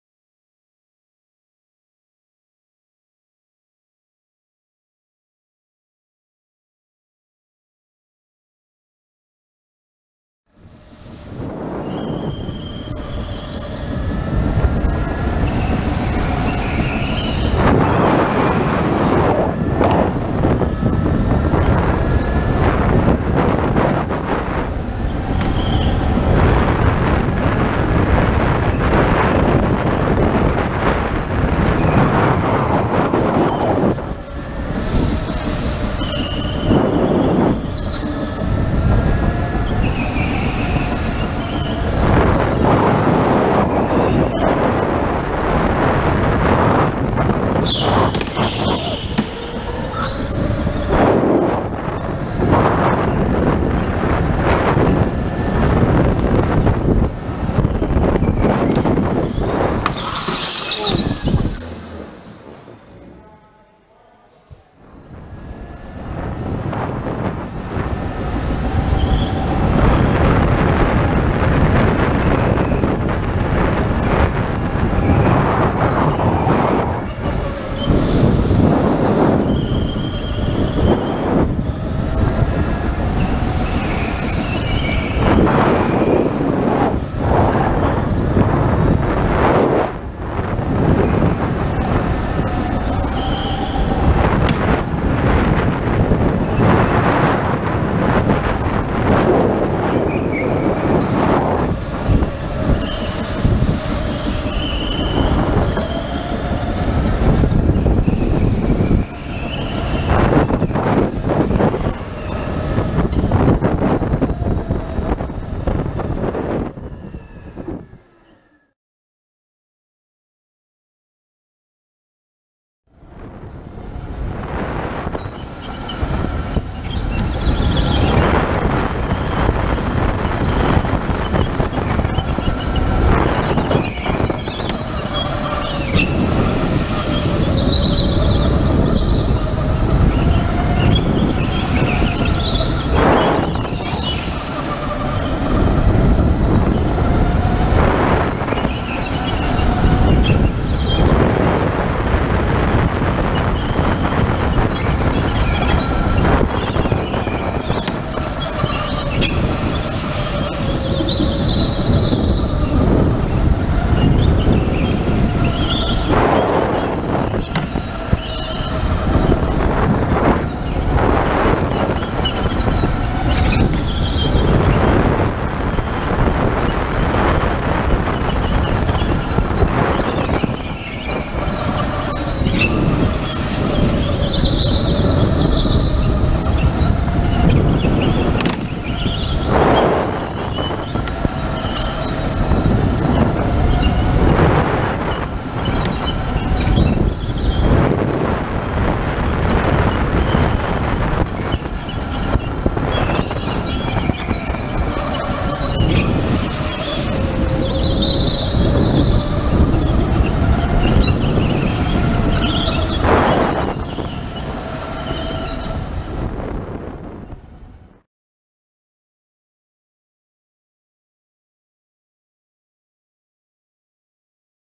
JD Racing Indoor Karting
Hit this track with my buddy, probably my third time there.  Fun little track, electric karts seem pretty consistent, no big gaps in performance.  My buddy is driving in the beginning of the video, and I come in for the second half of the video.

Lap times in the video range from 23.2s to the low 24's.